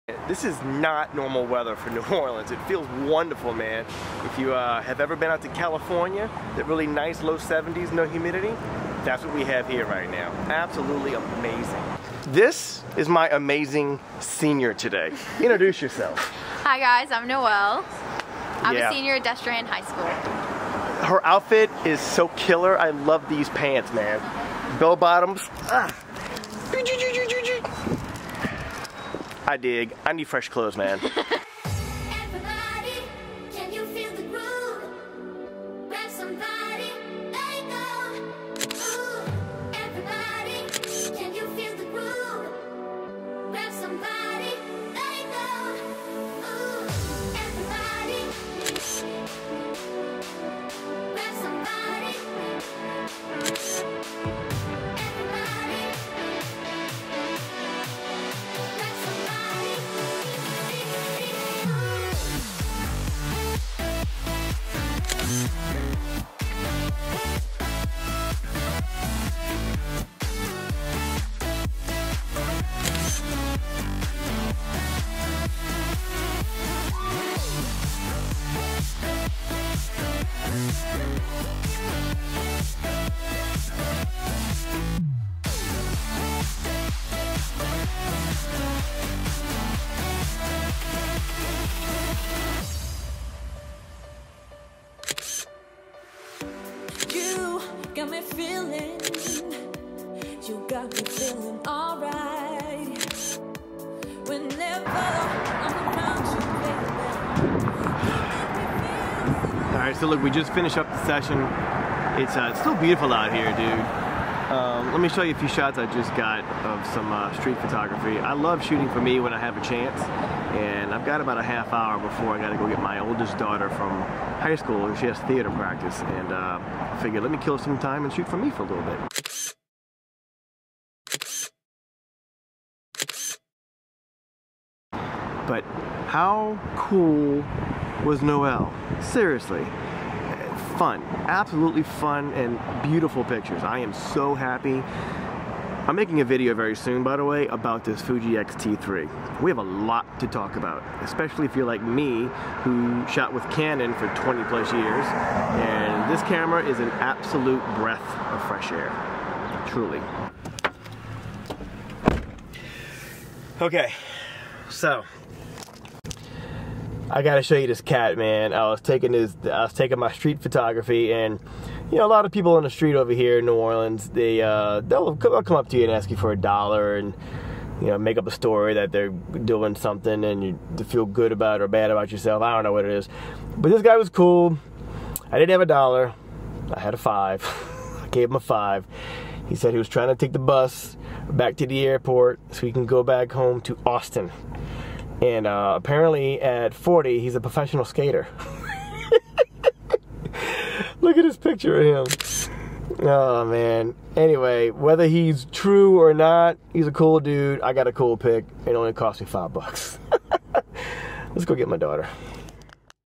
High School Senior Casuals - New Orleans- French Quarter
What a blast I had capturing High School Senior Casuals with  Noelle!  Watch the video and enjoy New Orleans as the background.

Code: JI0BCIVM